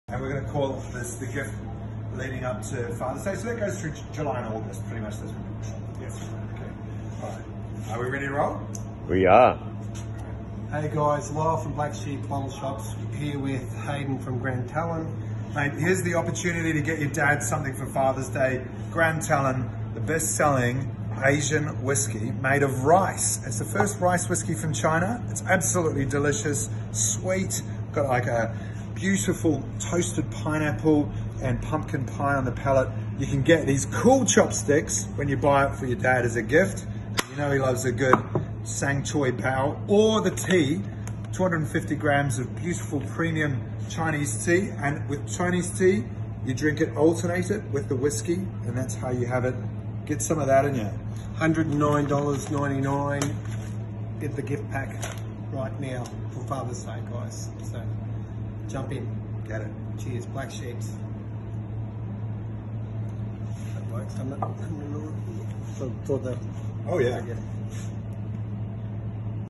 Let's have a taste! promo deal in Black Sheep bottle stores in Queensland!
Welcome to free taste Grand Talon Rice whisky in Queensland Black Sheep bottle store.